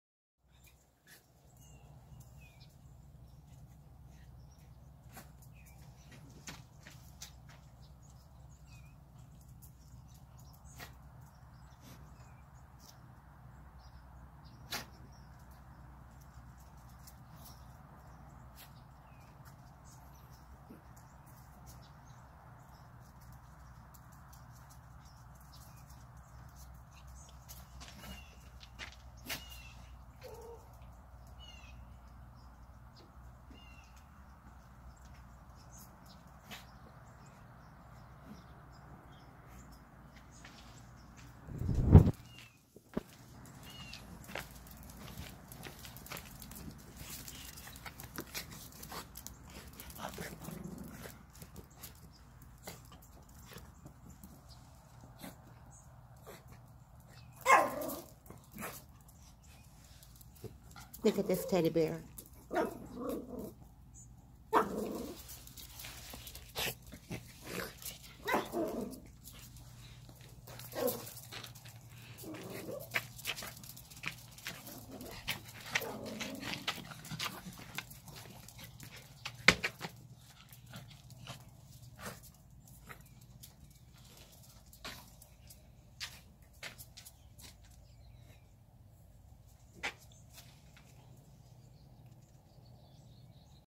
OUT DOORS DIY SANTA YARD ART
Hand painted Santa scarp wood all paint supply purchased at walmart